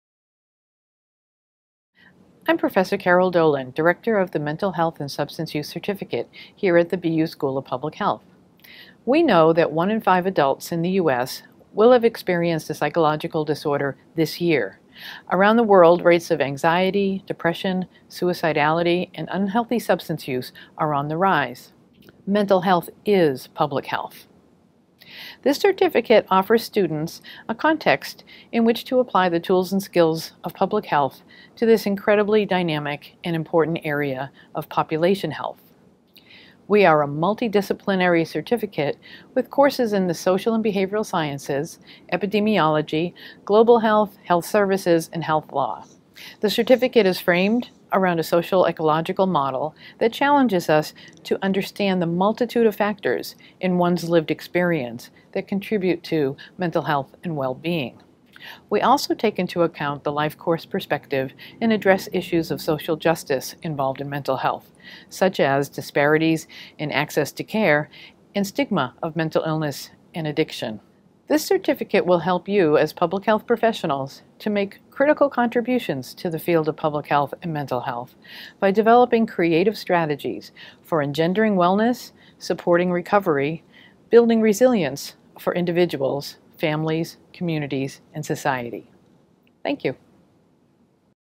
Mental Health and Substance Use Certificate at Boston University
Build the conceptual frameworks, knowledge, and technical skills necessary to understand how mental illness and substance-use disorders develop, are manifested, and are treated.